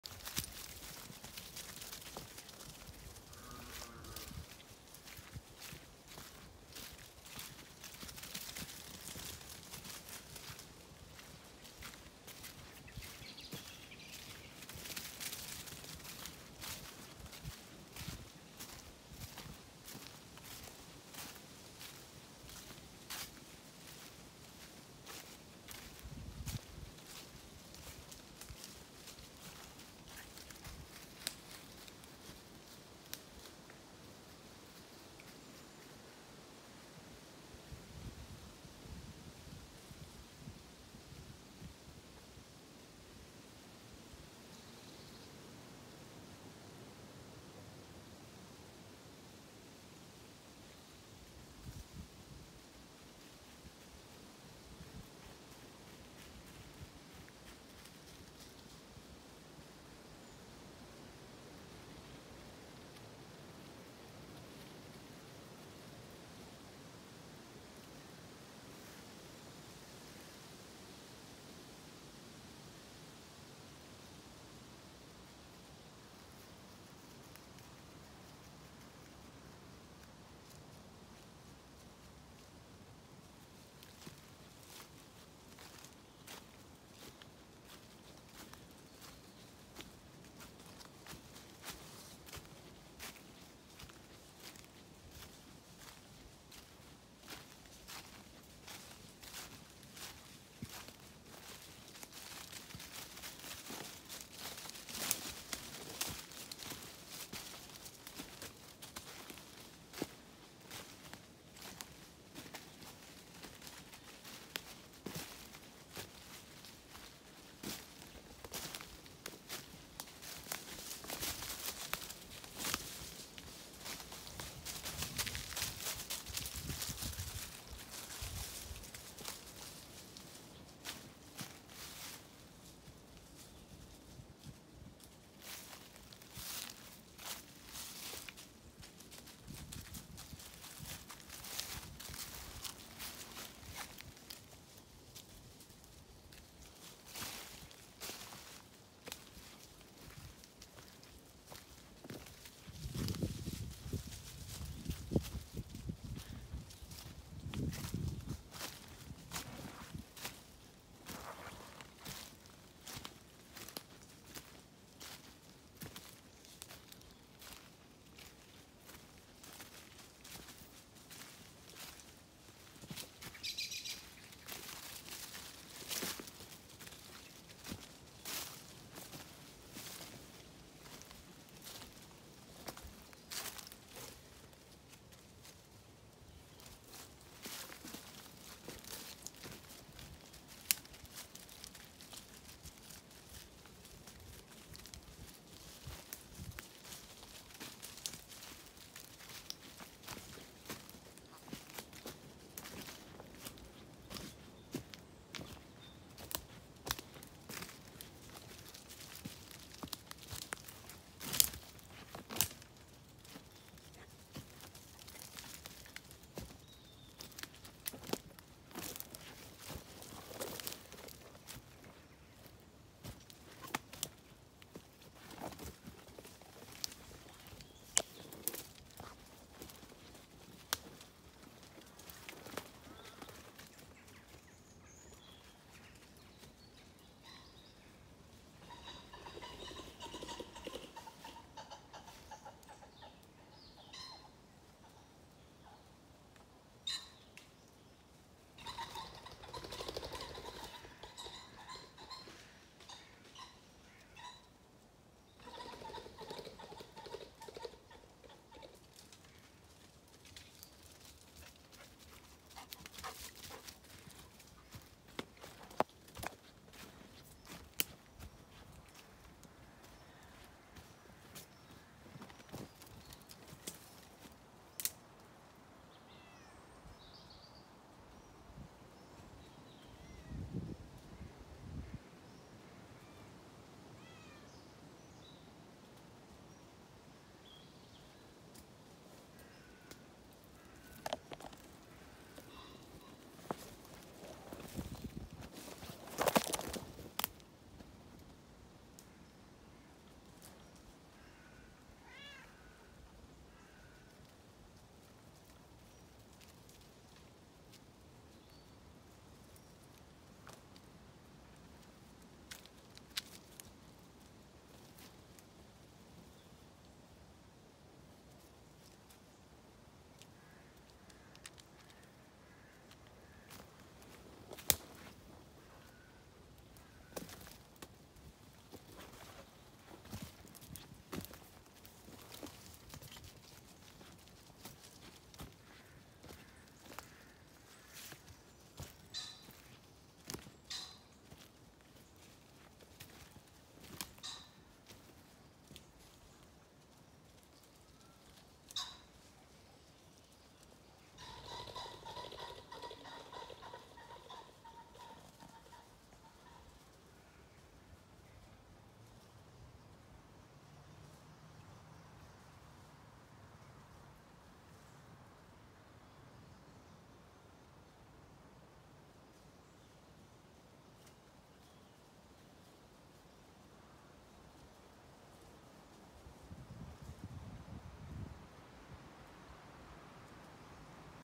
A Walk in the Woods 15 November, 2020. Sheep have cleaned ground cover
Ewes have now prepared the woods for spring woodland flowers to sprout and bloom. Their job is done now for this year of maintaining woodland biodiversity.